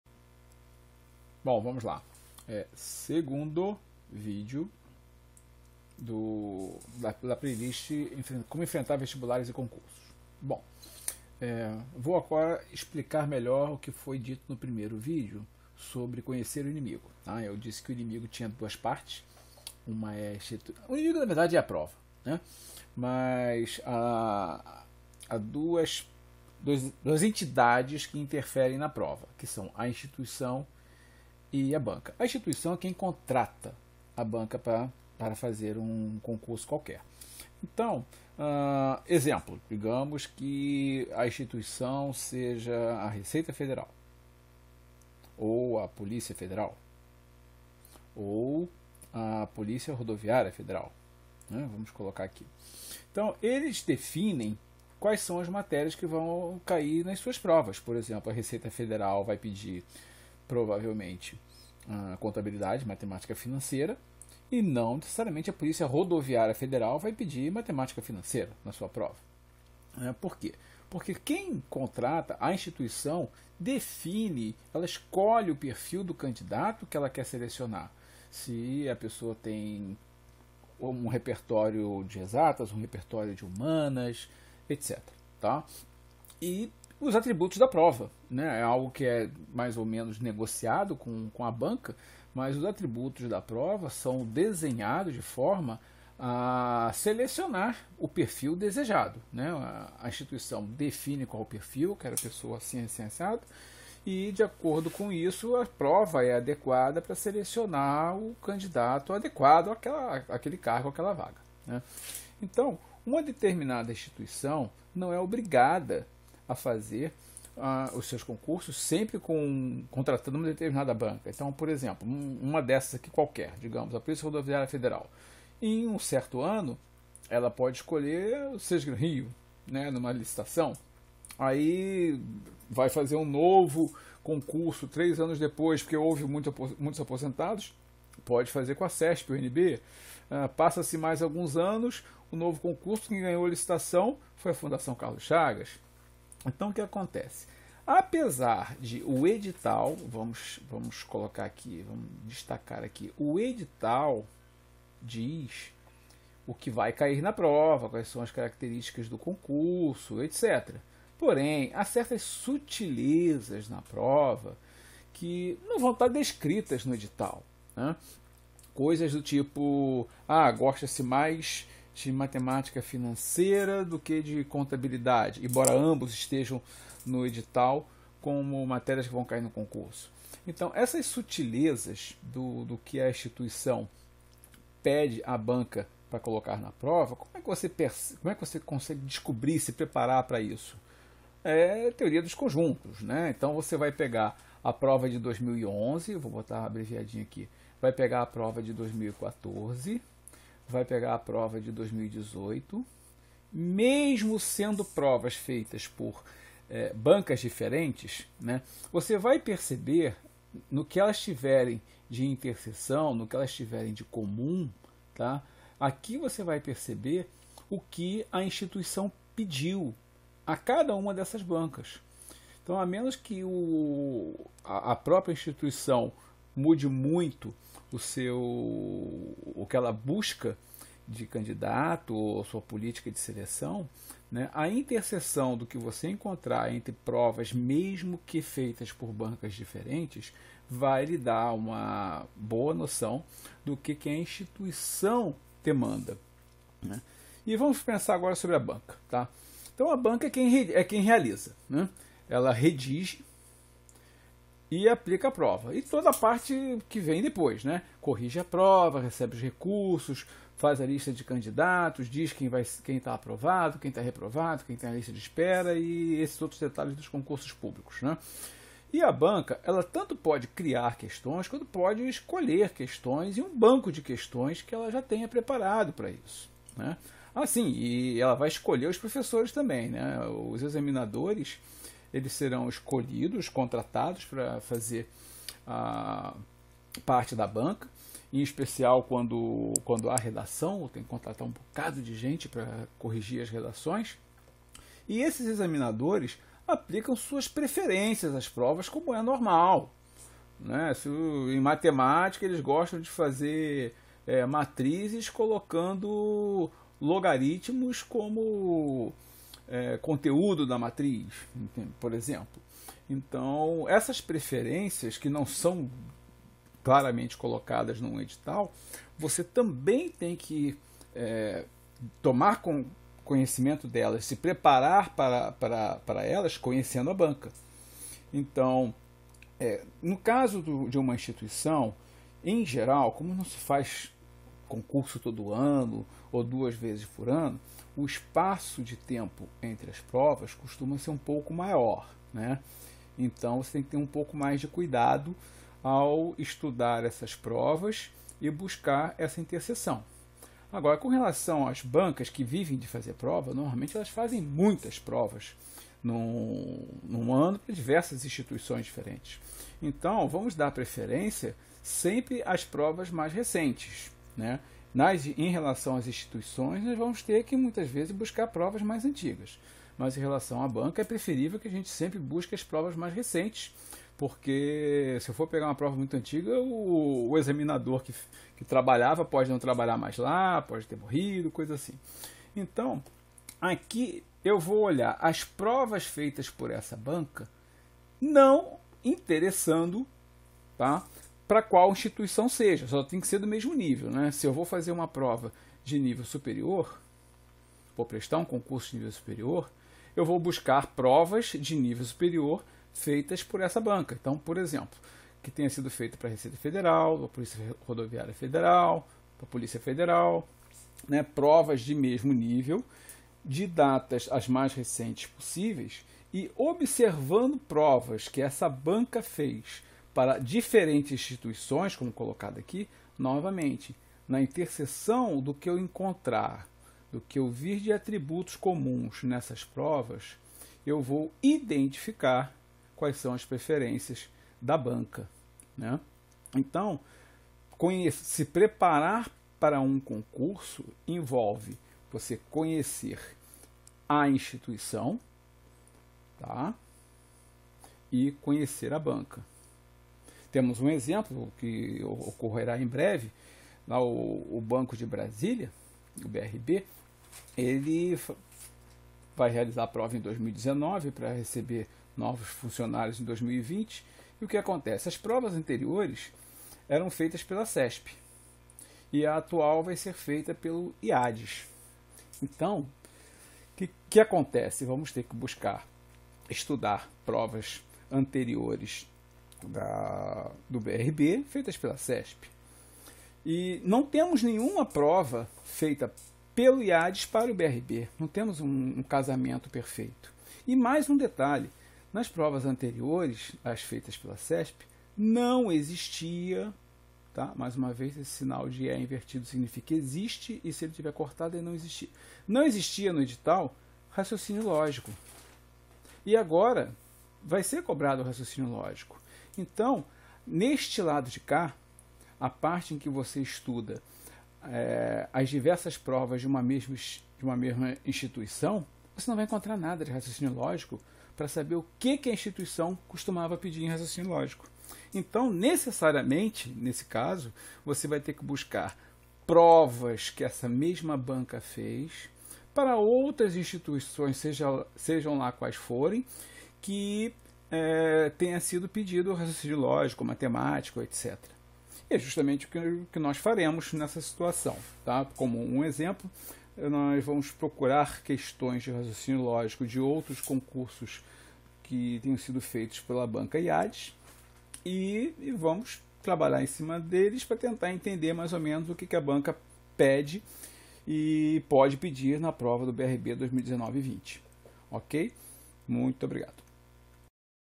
Como Enfrentar Vestibulares e Concursos 2
Assista o primeiro vídeo!

Neste vídeo são analisadas as influências da instituição que promove o concurso e da banca contratada para realizá-lo.